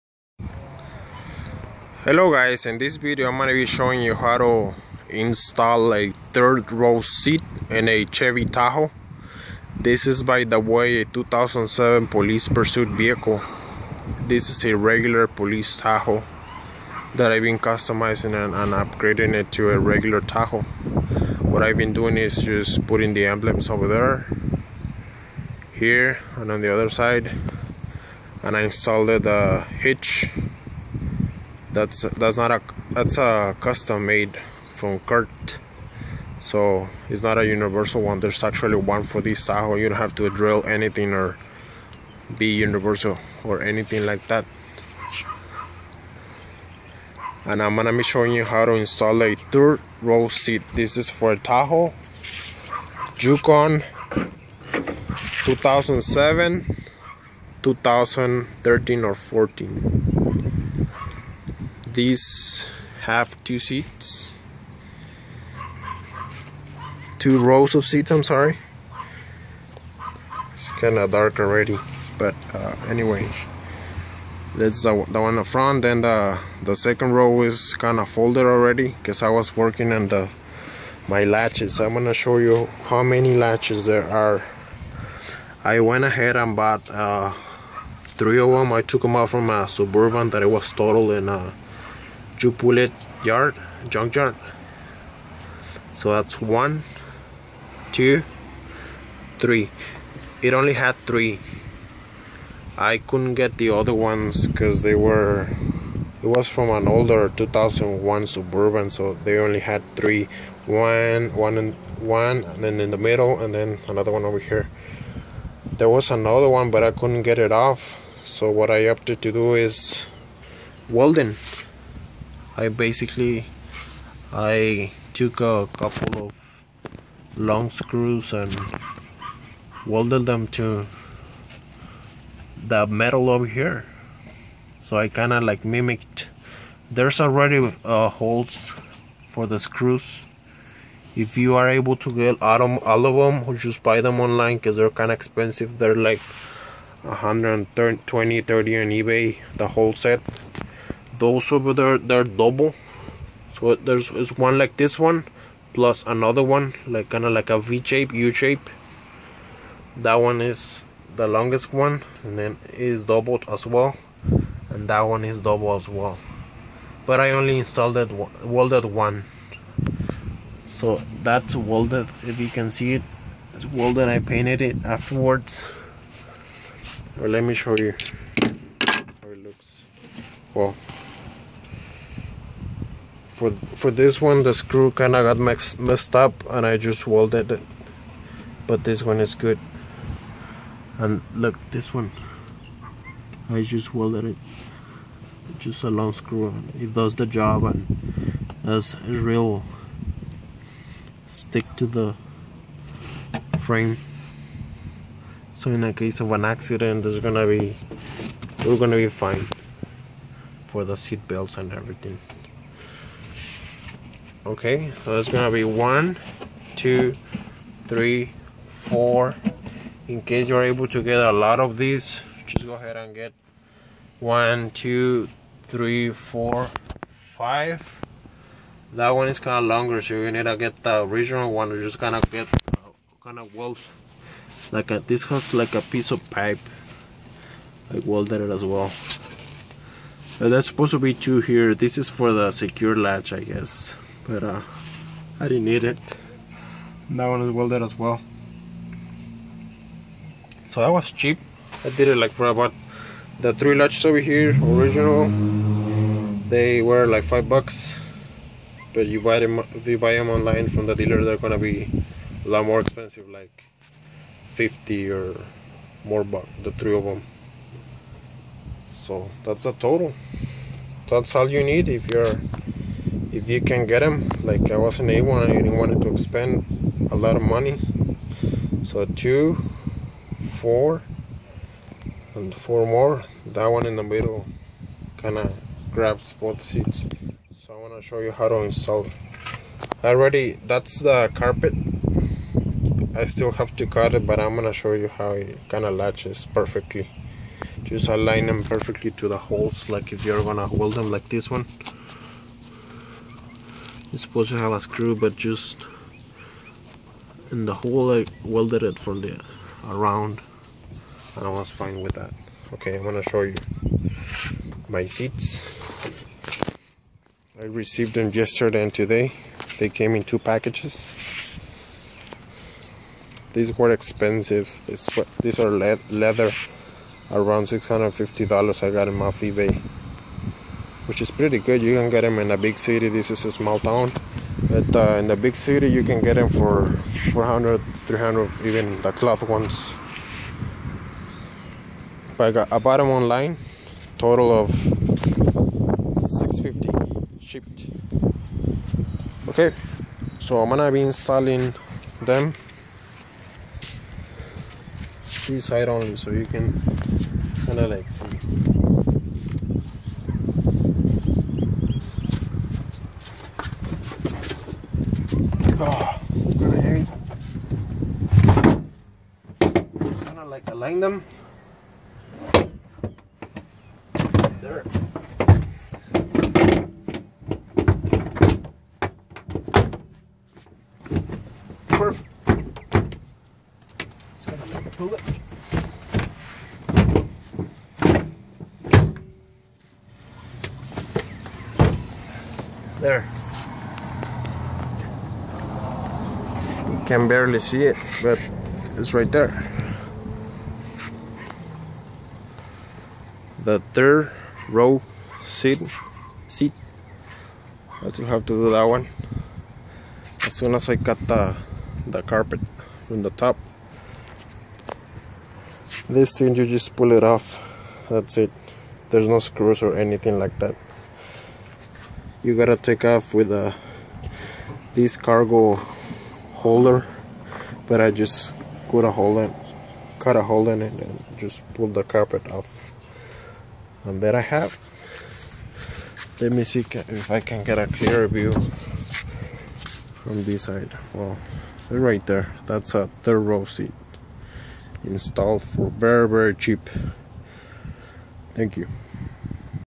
How I installed 3rd row seats Tahoe 2007-2013
My video shows how I installed my 3rd row seats in my 2007 Chevy police ppv Tahoe using partial brackets out 9th a boneyard and bolts welded down the floor for perfect latch and fitting. The third row